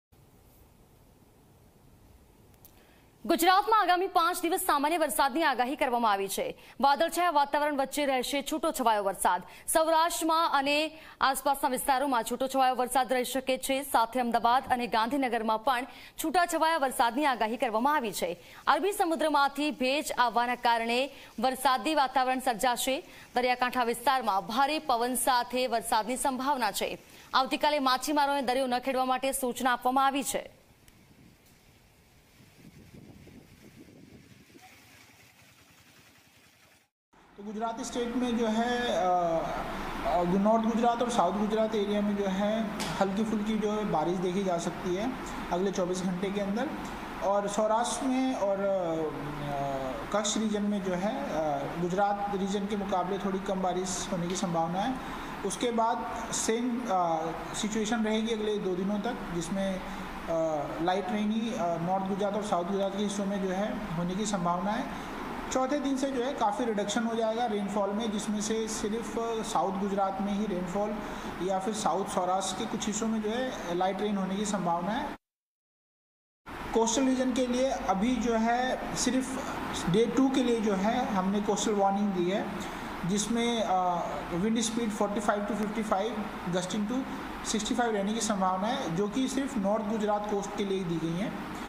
Light to moderate rains along with gusty winds predicted in parts of Gujarat for next 5 days: IMD
ગુજરાતમાં આગામી 5 દિવસ સામાન્ય વરસાદની આગાહી
વાદળછાયા વાતાવરણ વચ્ચે રહેશે છુટોછવાયો વરસાદ
સૌરાષ્ટ્રમાં છુટાછવાયા વરસાદની શક્યતા
અમદાવાદ અને ગાંધીનગરમાં પણ છુટાછવાયા વરસાદની આગાહી--
અરબી સમુદ્રમાંથી ભેજ આવવાના કારણે રહેશે વરસાદ---
દરિયાકાંઠા વિસ્તારોમાં ભારે પવન ફૂંકાવાની સંભાવના---
આવતી કાલે માછીમારોને દરિયો ન ખેડવાની સુચના